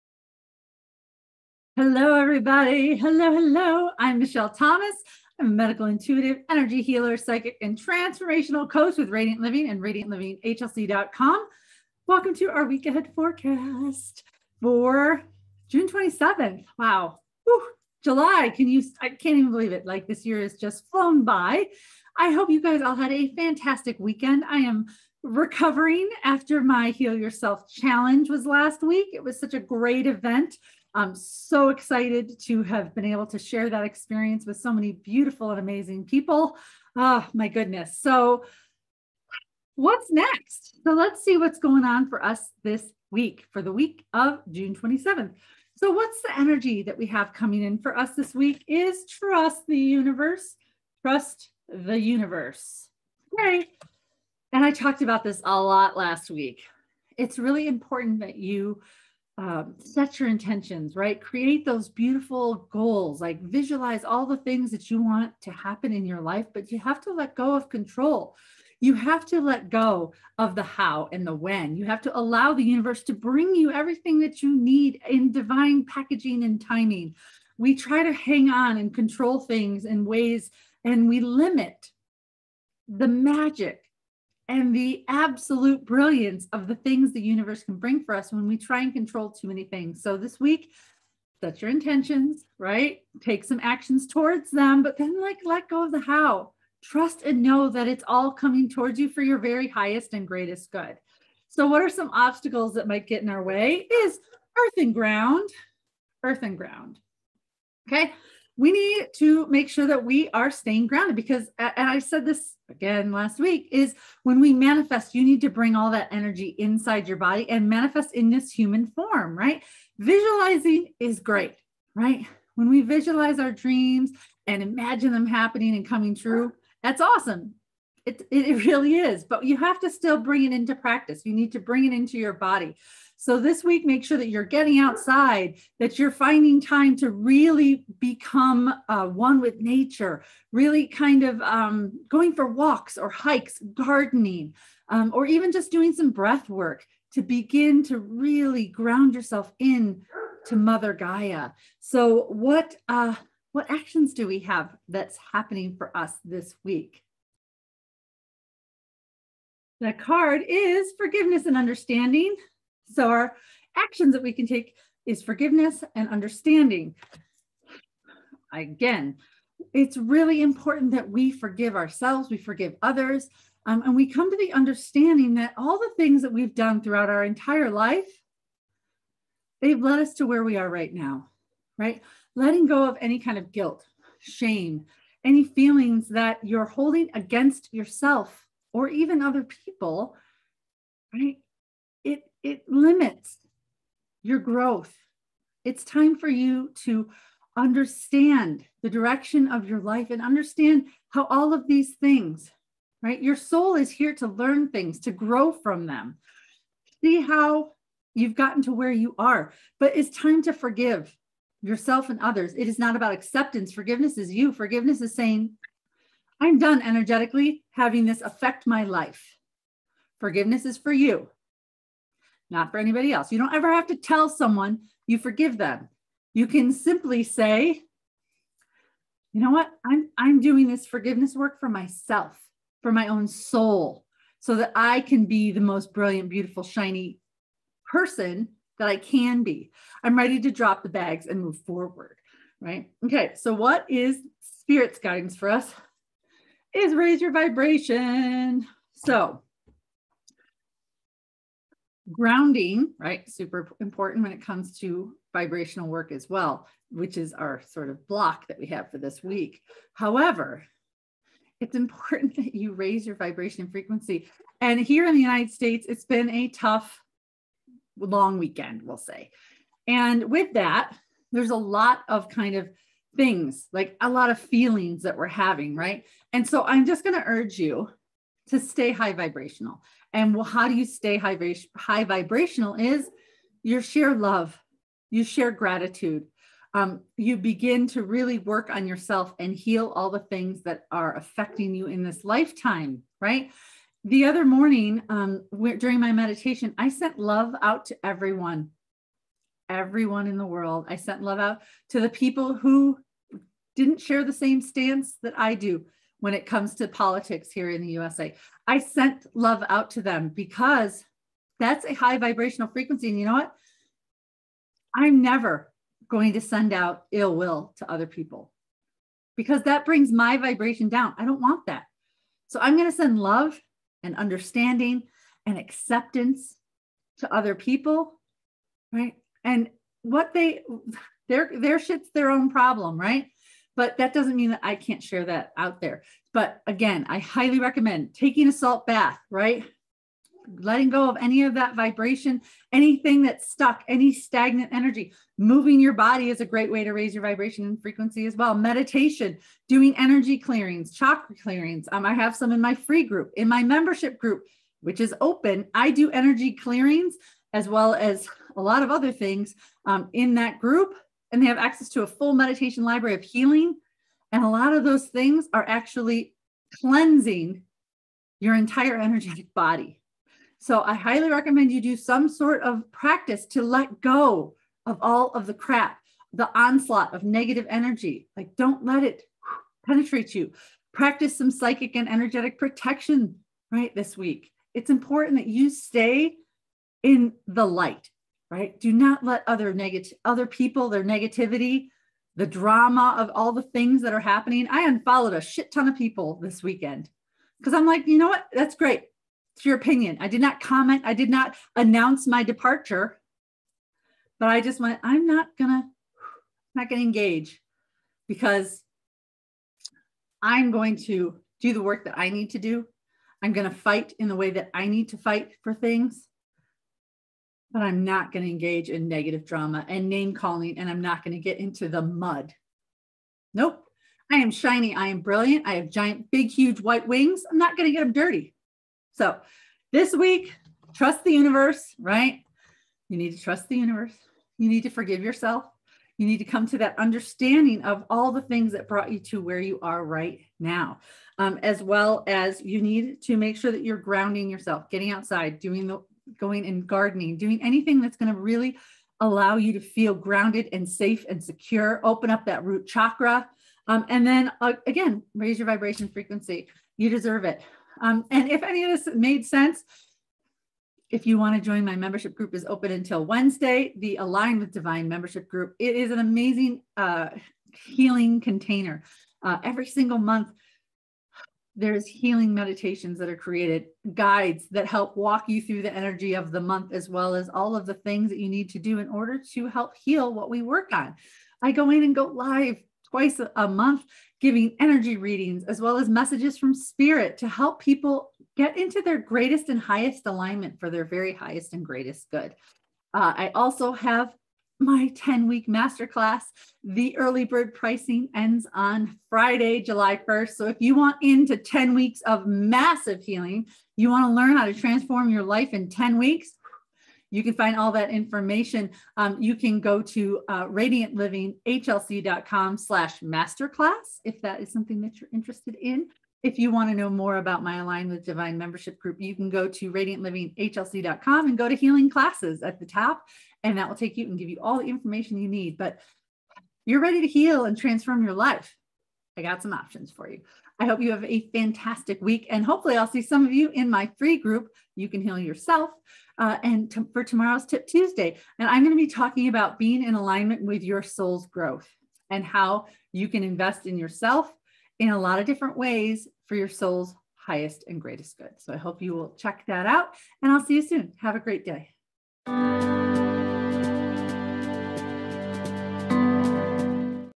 💫 Week Ahead Oracle Forecast for June 27th, 2022
💫 Week Ahead Forecast - June 27th
💫 Energy: Trust the Universe
💫 Obstacles: Earth and Ground
💫 Actions: Forgiveness and Understanding
💫 Spirit's Guidance: Raise your Vibration

Happy Monday!
I'm still recovering from the amazing Heal Yourself Challenge. It was such a wonderful experience sharing healing with those who participated! I'm so grateful to everyone who joined.

So what do we have in store for the week?
I spoke a lot about the need to relinquish control last week during the Challenge when it comes to our intentions and dreams. The universe is capable of bringing you something infinitely better than what you could even imagine. So stop limiting how the universe brings you the people, resources, and opportunities in your life. You are divinely supported if you allow yourself to be!

It is a great week for getting outside and being at one with nature and mother Gaia. Go for a hike, do some gardening or breathe work to being all that scattered energy back into your body. Remember, manifestation happens in the physical form. Being grounded in an essential part of that.

This week, it's also a great time to understand your journey.  Seeing all the paths and choices that led you to where you are right now. Forgive yourself and others where needed to release the blocks and baggage you've been carrying around for far too long. Remember, forgiveness is for you and ONLY you.  It helps you release the energetic link between you and that other person or experience.

It's also a time to raise your vibration and frequency this week. With the New Moon on Tuesday, it's a great time to take a salt bath, meditate, go for a walk, eat nourishing foods, focus on sharing more love and gratitude with the world, and move your body to remove stagnant energy. The world needs us all to be as high vibrational as possible. Let go of negativity and don't engage in drama.

Hope you have a fantastic week.
Remember, my membership is open for new members through Wednesday. If you grab a spot today, you even get some messages from spirit later today when I'm live in the group!oraclecards